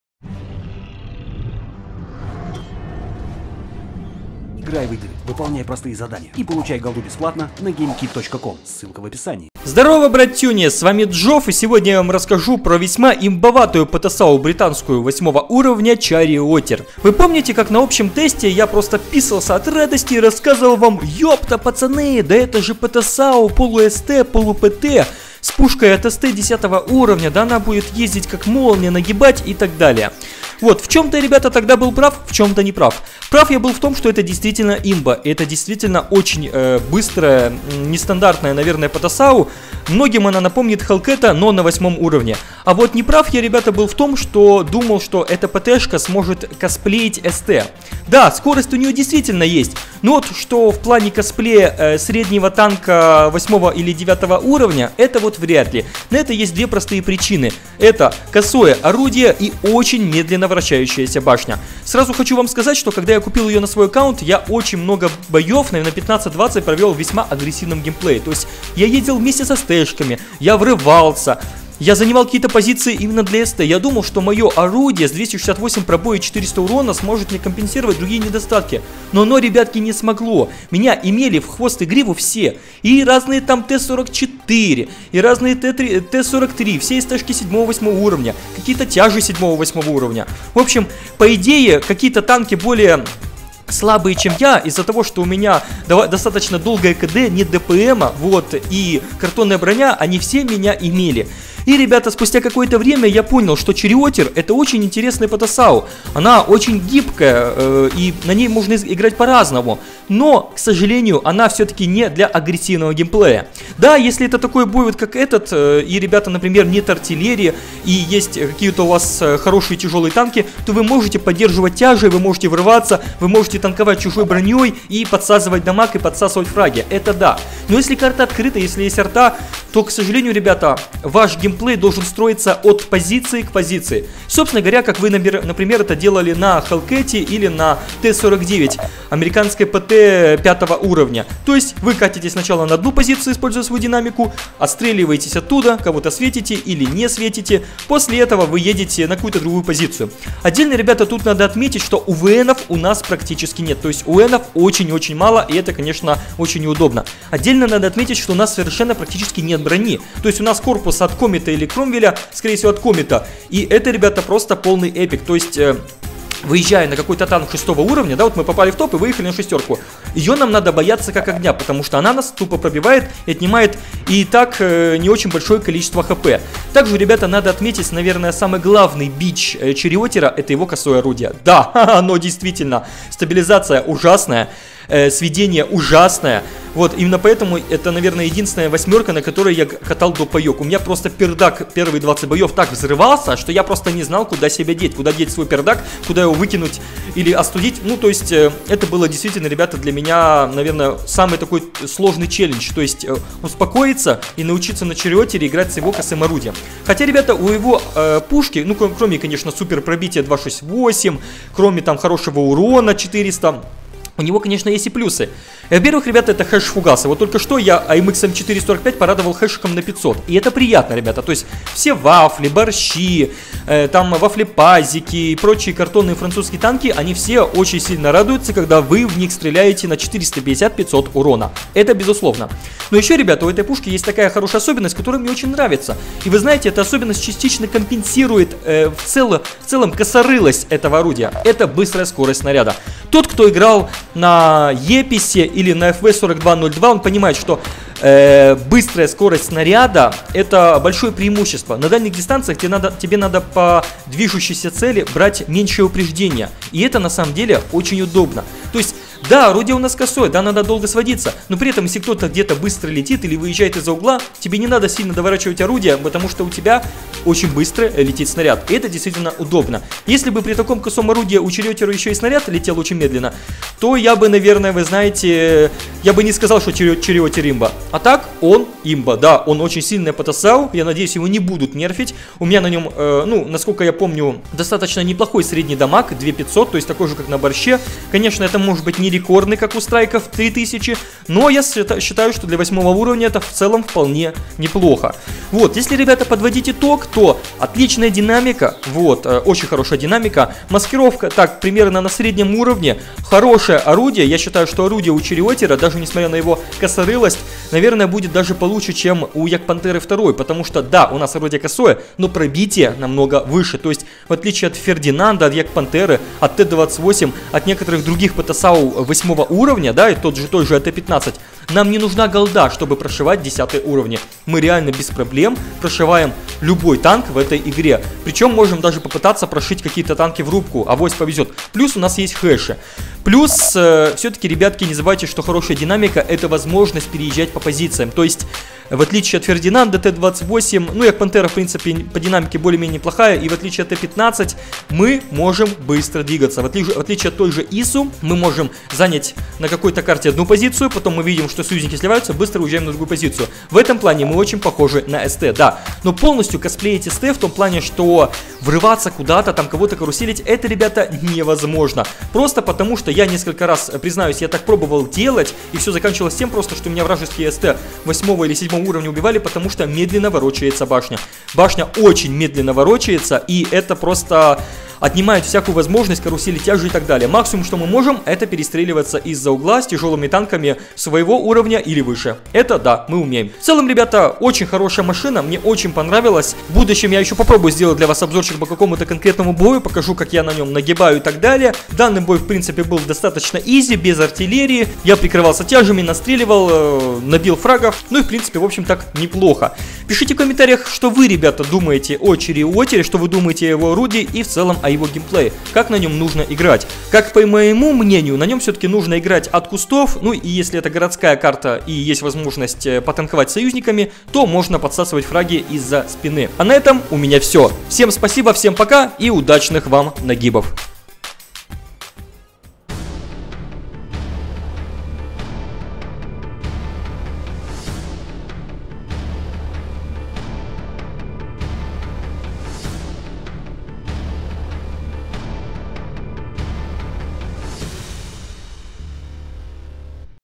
Кривая Имба! (Обзор Charioteer)
Что могло быть лучше старого М18 Hellcat? Только такая же машина, но уже на 8 уровне и с пушкой от СТ10.

Представляю вам Charioteer - британскую ПТ с замашками СТ. Полноценно рашить и беспредельничать на ней не получится, но крысо-читерить из кустов и набивать дофига урона - вполне!